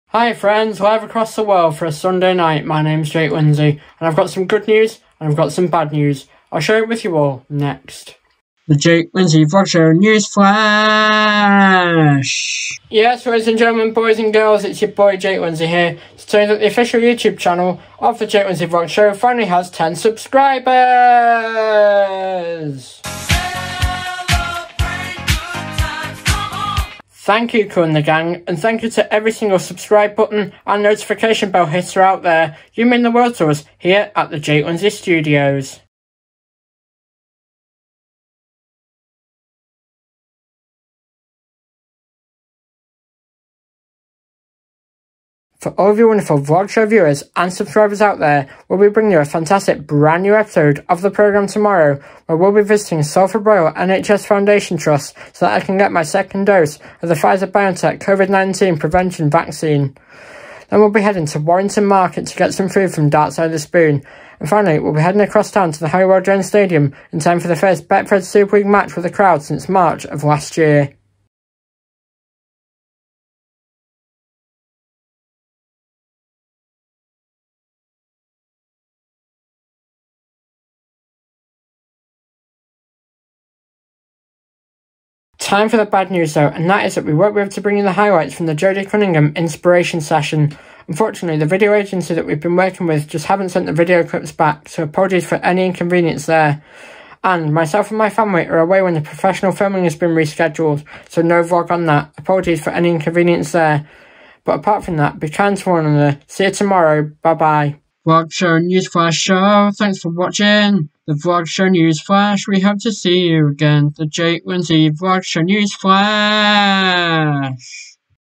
THE JAKE LINDSAY VLOG SHOW NEWSFLASH EPISODE 1: 10 SUBSCRIBERS
Here is an update all about this very channel hitting a whopping 10 subscribers

I hope that you enjoy it as much as I did making and uploading it

Cheers, see you all tomorrow

Jake